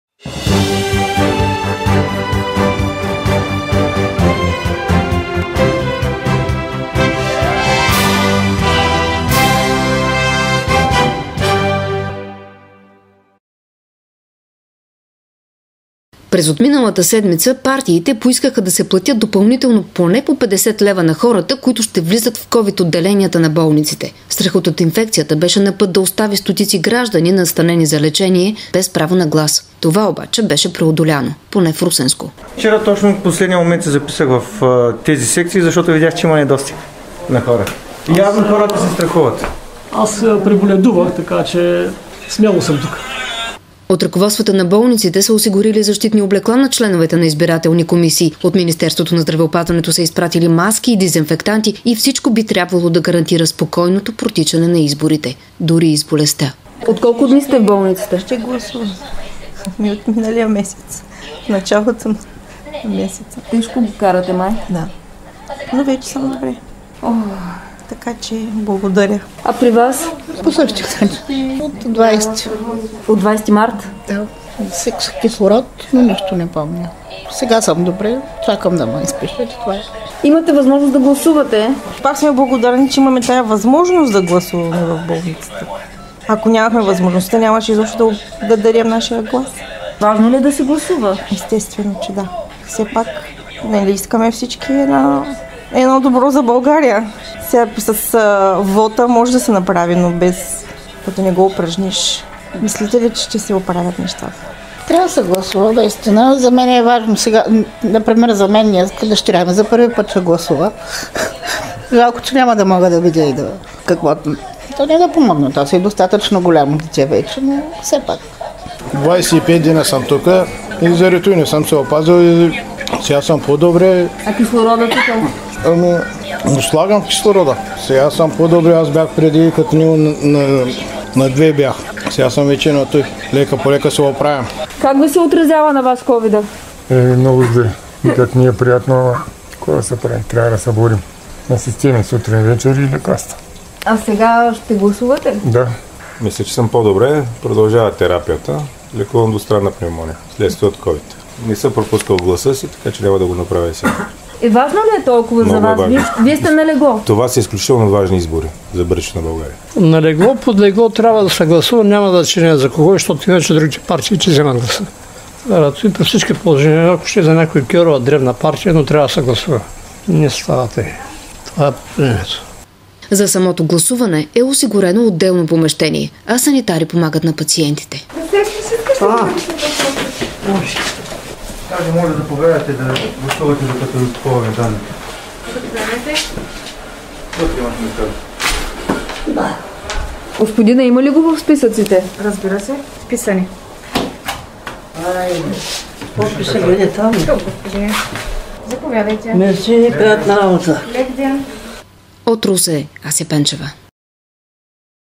Как се гласува в COVID-отделениe
Хората, които се лекуват в COVID-отделенията на болниците също имат право на глас. Ето как протича гласуването там.
Репортаж от Русе на Ася Пенчева